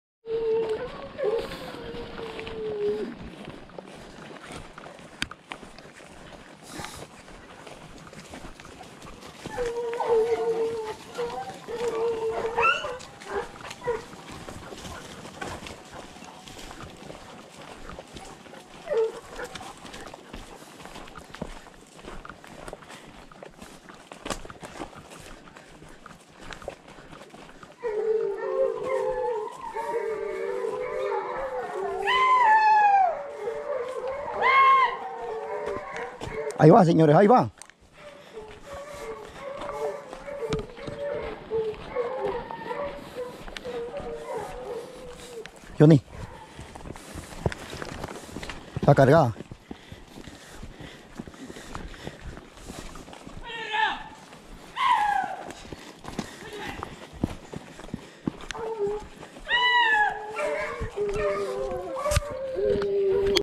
Carrera de Venado
Los cachorros aprendiendo a correr el venado.
pura adrenalina.